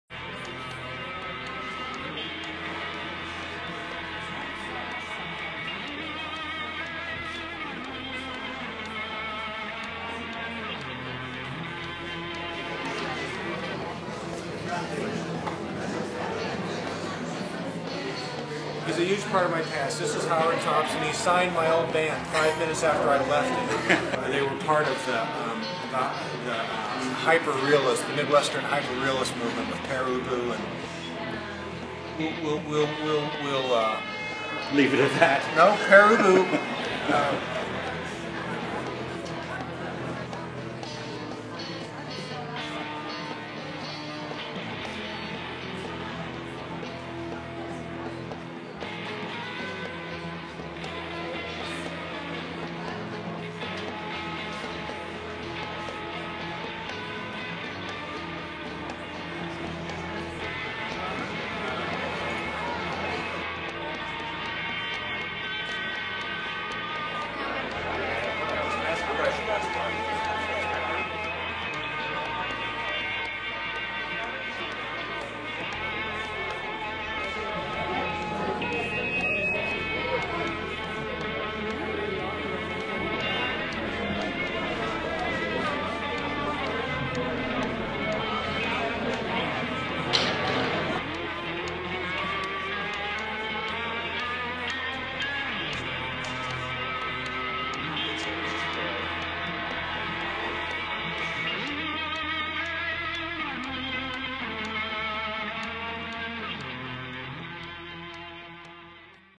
7/7 - Kevin Teare - "Upon This Rock" Interview - (Opening Reception)
Kevin Teare, "Upon This Rock" Opening Reception
Guild Hall, East Hampton, NY, December 5, 2009